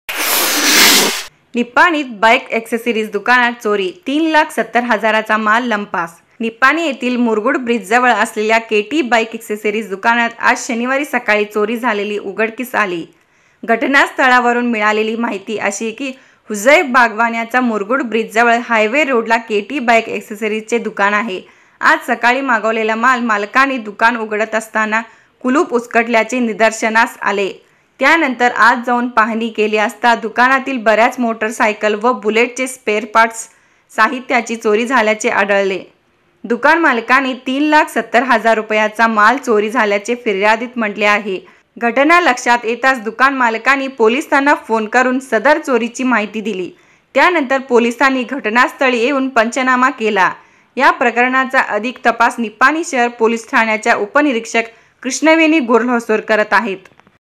निपाणी मध्ये चोरीचे सत्र सुरूच : बाईक अक्ससिरिज दुकानात 3लाख 70 हजार रुपयेचा माल चोरी
निपाणी मध्ये चोरीचे सत्र सुरूच : बाईक अक्ससिरिज दुकानात 3लाख 70 हजार रुपयेचा माल चोरी  (ICB NEWS NIPANI)